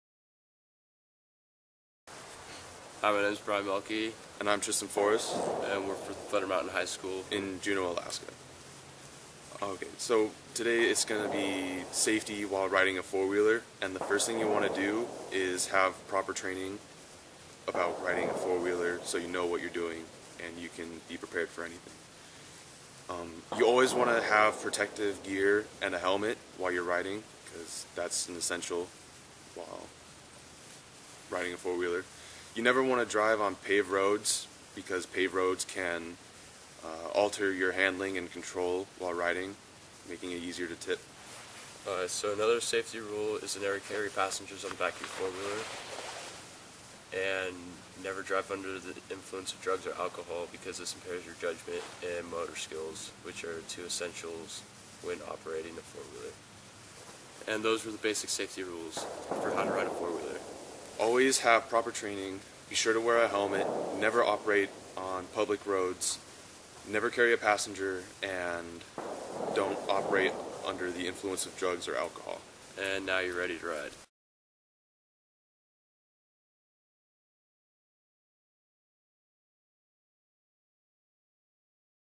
Fourwheeler safety
A safety movie I made for my mining class